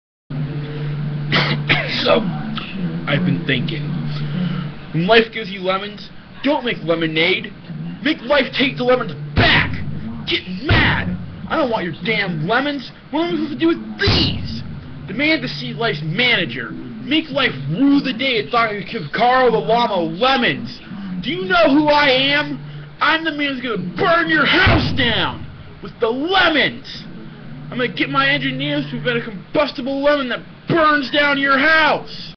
Carl the llama rants about Lemons.
Portal 2 reference.